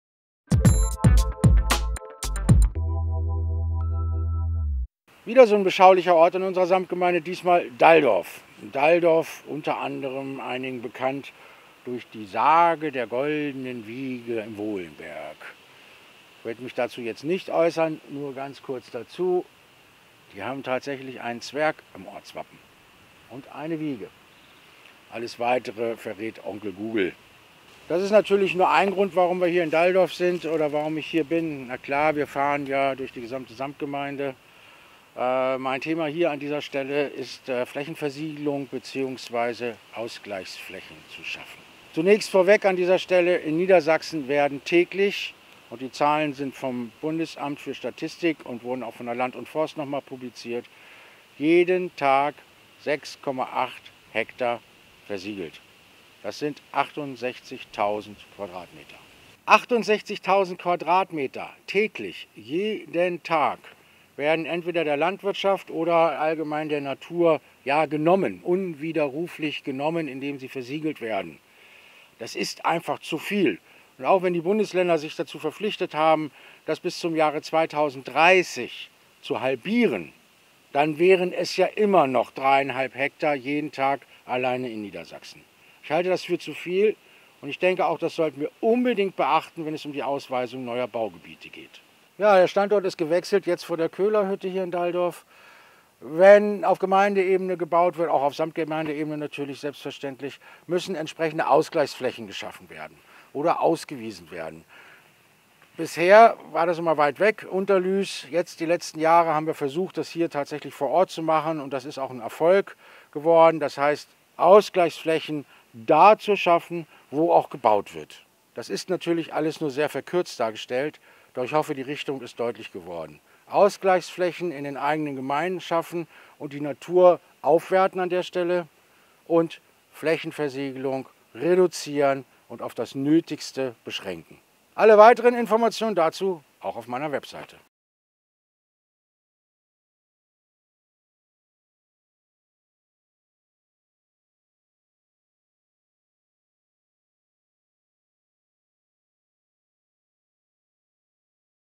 Pesi Daver in Dalldorf
In Niedersachsen werden täglich 6,8 ha Fläche versiegelt. In den betroffenen Gebieten unserer Samtgemeinde sollten vorgesehene Ausgleichsflächen ortsnah angelegt werden und die Natur aufwerten.